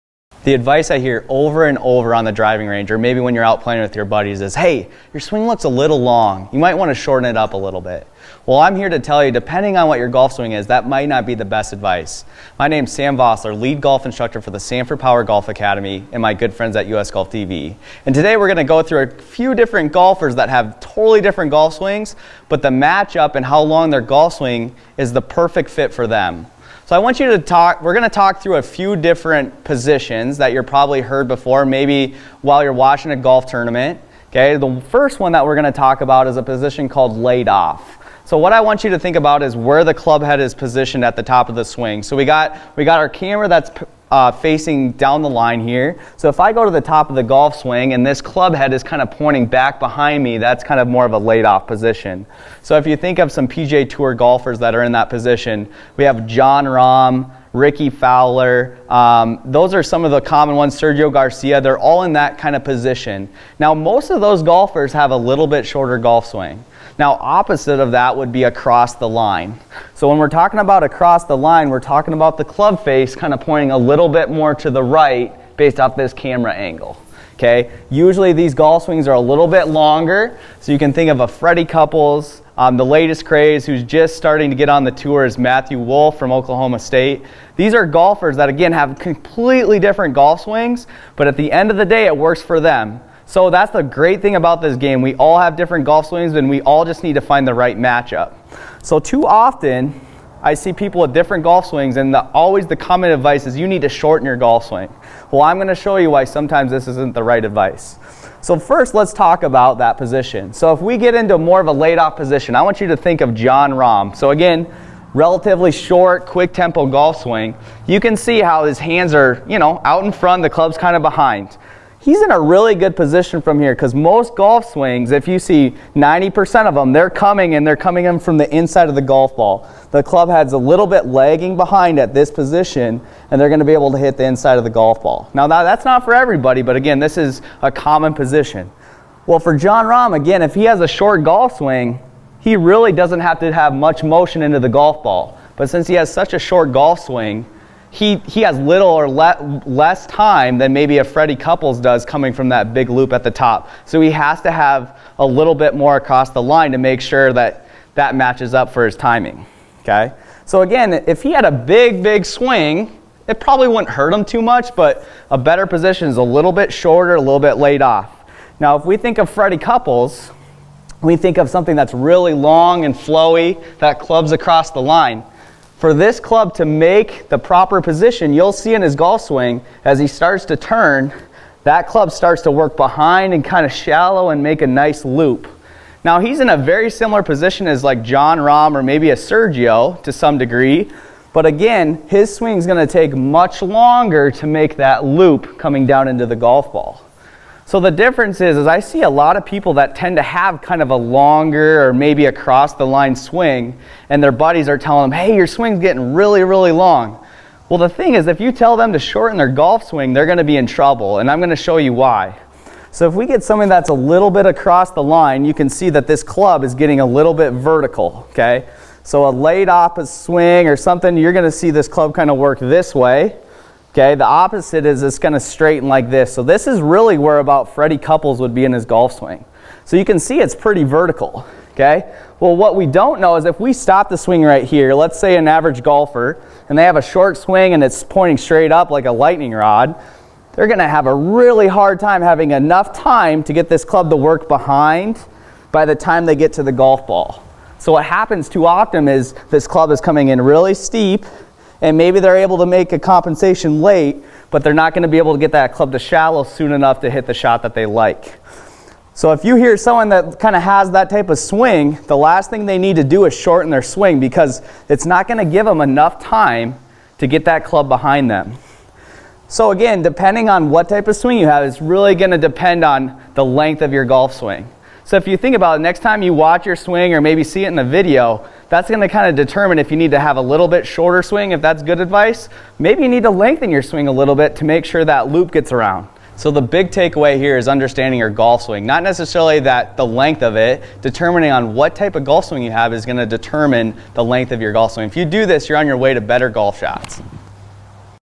Golf Swing Length: Should Your Backswing Be Short or Long?
One of the most common things we hear on the golf course is that our golf backswing is too long, or that our swing needs to be shortened.  Unfortunately, depending on your type of golf swing, that may not necessarily be the truth.

If you have more of a Jon Rahm swing that is laid off, you might want to shorten your golf backswing. Likewise, if you have more of a Fred Couples golf swing that comes across the line in the backswing, you will probably want a longer swing to be able to get that downswing in the slot.

Sanford POWER Golf Academy's Lead Instructor Sam Vosler shows us that if you have a laid off golf swing or a swing that is more across the line, that will determine the real length of your golf backswing.